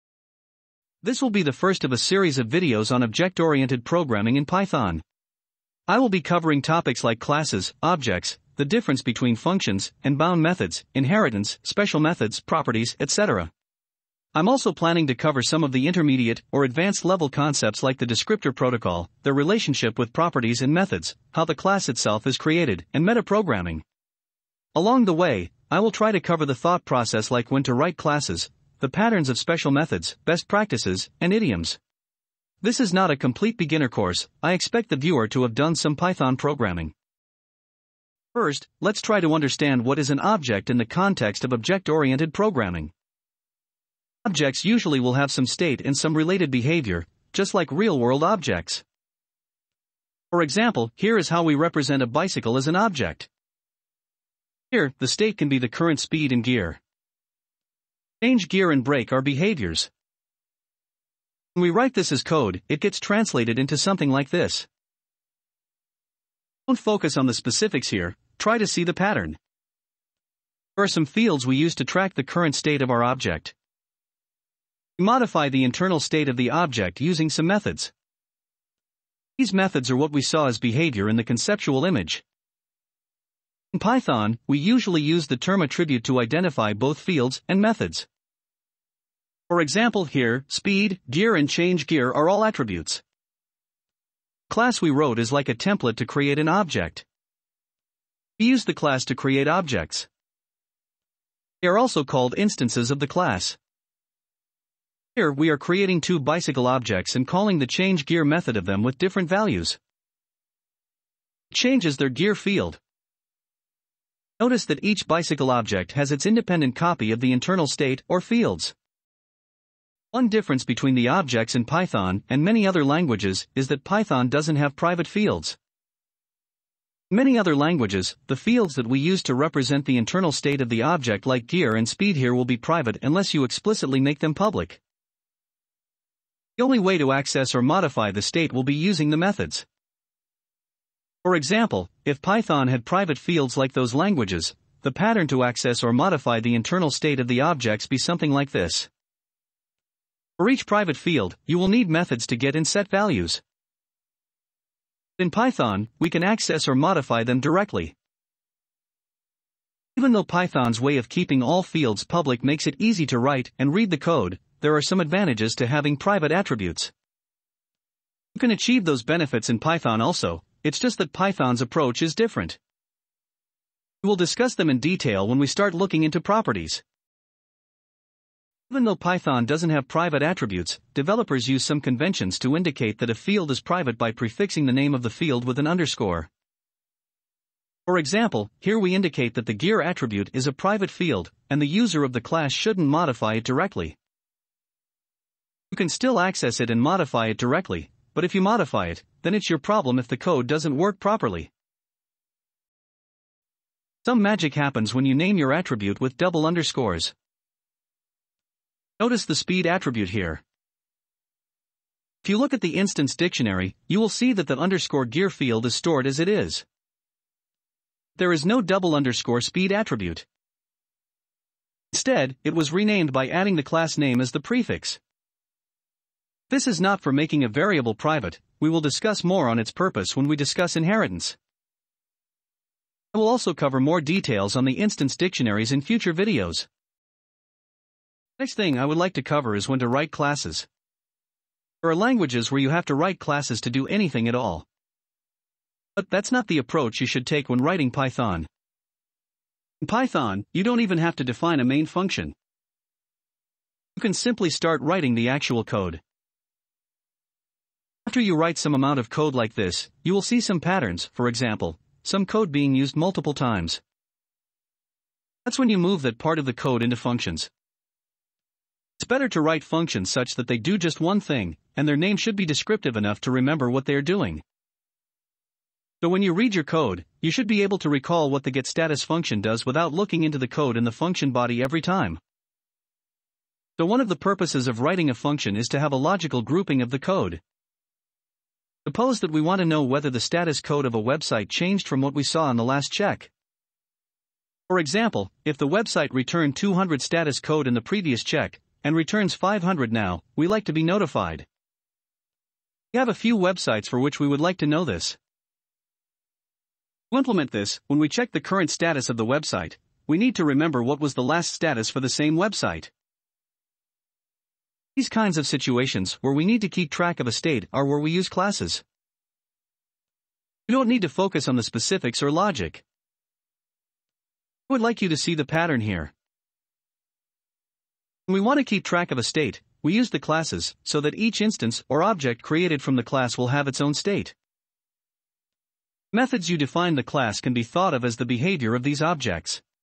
Object-oriented Programming in Python - part 1 [Introduction]
Introduction video for an intermediate level series on Object Oriented Programming in Python.

0:00 Introduction
0:38 OOP conceptual diagram
1:00 Translating concept diagram to code
2:00 Private fields
2:58 Conventions
3:23 field name mangling
3:57 Patterns of writing programs in Python

Credits: The contents of this video are mostly from my notes on Dr. Fred Baptiste's course